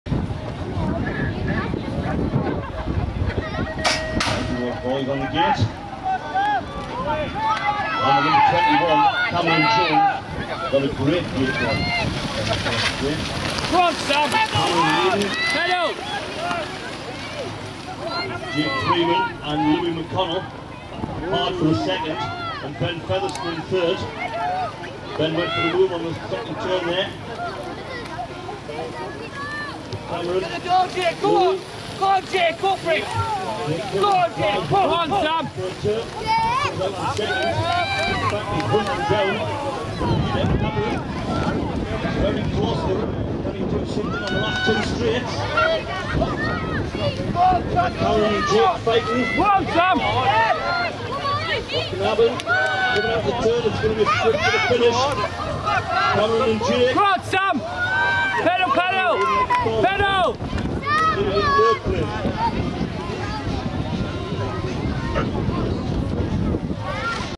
Male 12 Moto 2 Gate 1 (North SS11-Rnd6).mp4
North Region BMX Summer Series 2011 - Round 6 at Hartlepool BMX Track on the 26th June 2011.

This footage was filmed by Moley.